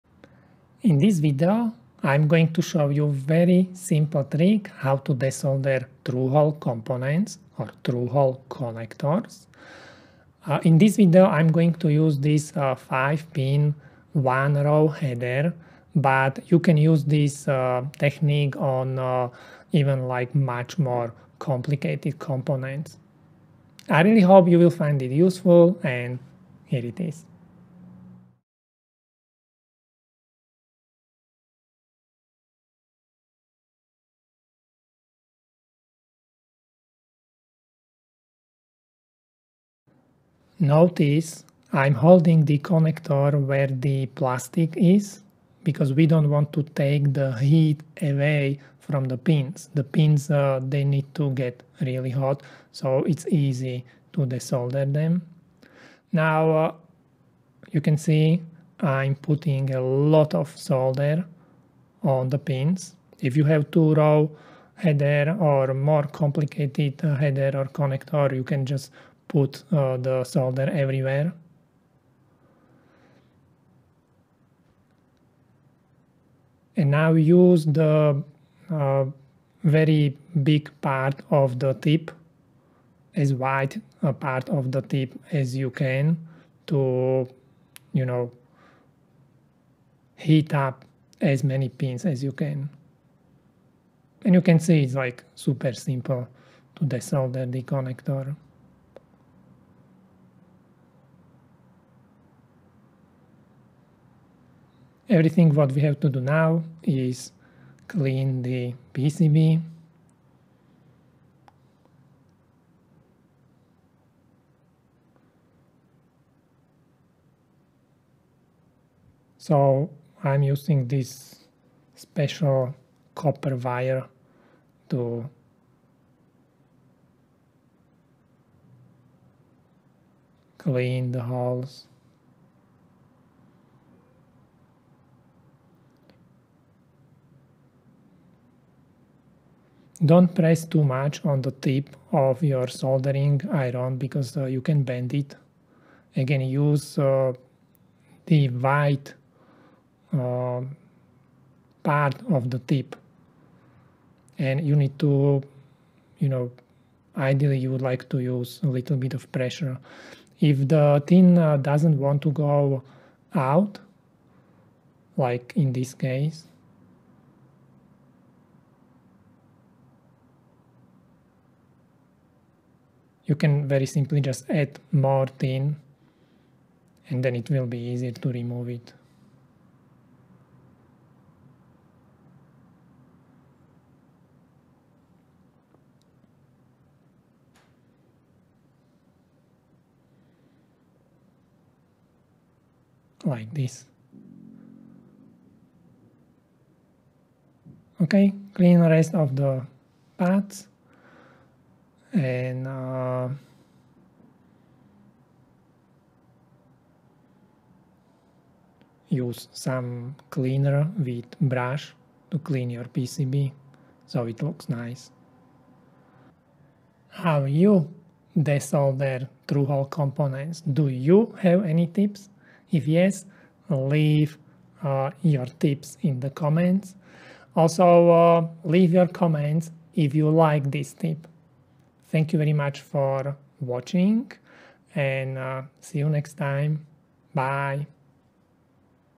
Easy Way to Desolder and Remove Through Hole Connectors or Components
Just a quick video about de-soldering components. I hope you will find it useful.

It is much appreciated. Thank you,
- Robert